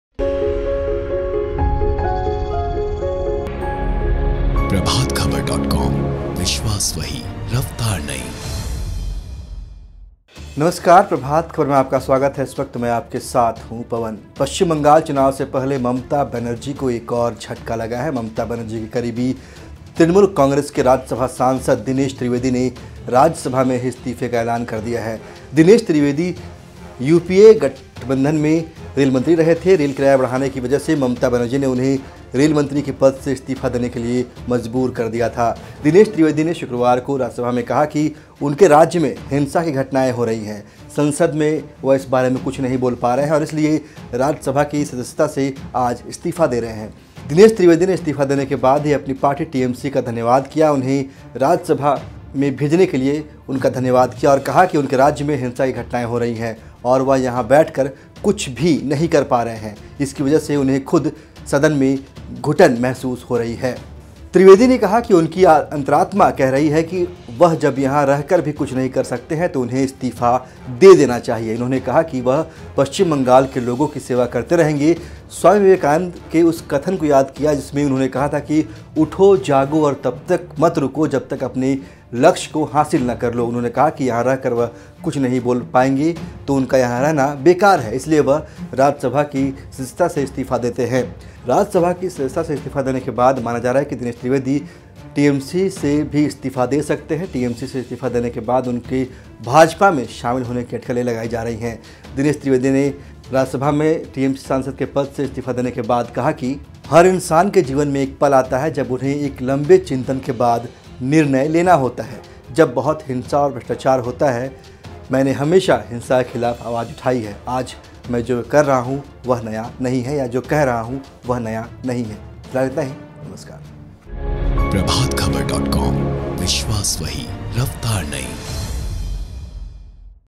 तो क्यों सदन में घुट रहे थे Dinesh Trivedi जो उठाया यह कदम I Dinesh Trivedi resigns from Rajya Sabha
पश्चिम बंगाल चुनाव से पहले ममता बनर्जी को एक और झटका लगा है. ममता बनर्जी के करीबी तृणमूल कांग्रेस के राज्यसभा सांसद दिनेश त्रिवेदी ने राज्यसभा में ही इस्तीफे का एलान कर दिया है. दिनेश त्रिवेदी संयुक्त प्रगतिशील गठबंधन (यूपीए) सरकार में रेल मंत्री रहे थे. दिनेश त्रिवेदी ने इस्तीफा देने से पहले अपनी पार्टी तृणमूल कांग्रेस का धन्यवाद किया कि उन्हें राज्यसभा में भेजा. कहा कि उनके राज्य में हिंसा की घटनाएं हो रही हैं और वह यहां बैठकर कुछ नहीं कर पा रहे हैं. इसकी वजह से उन्हें सदन में घुटन महसूस होती है.